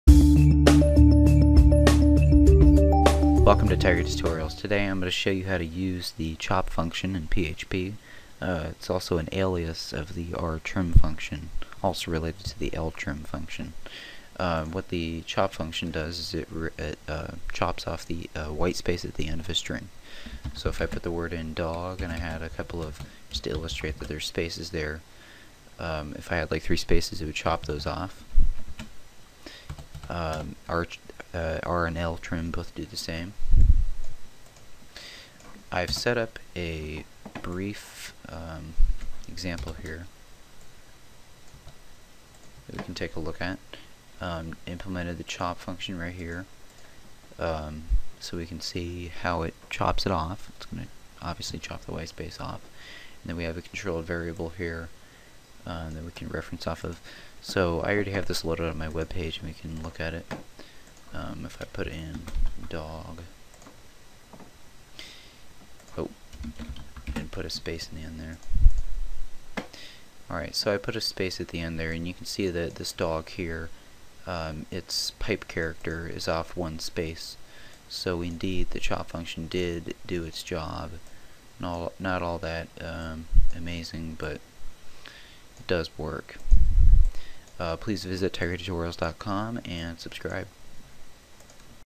PHP String Function: chop( )
Use the PHP String function chop( ) to remove whitespace from the end of a string.

for more tutorials!